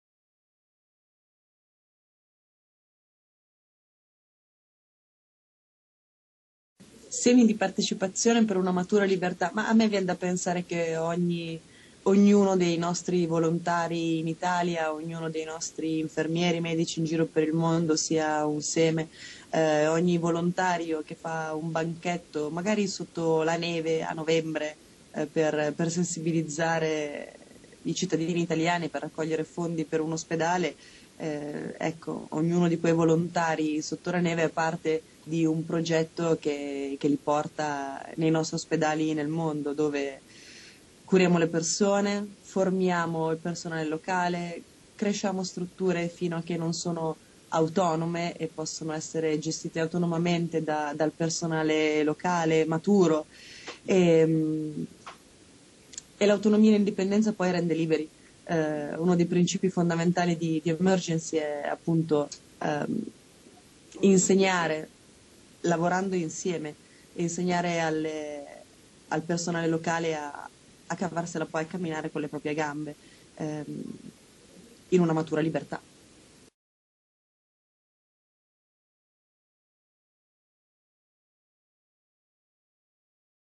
Il seme di partecipazione di Cecilia Strada
Il seme di partecipazione di Cecilia Strada, presidente di Emergency, al 51°Congresso Nazionale Pubbliche Assistenze (Roma 2-4 dicembre 2011)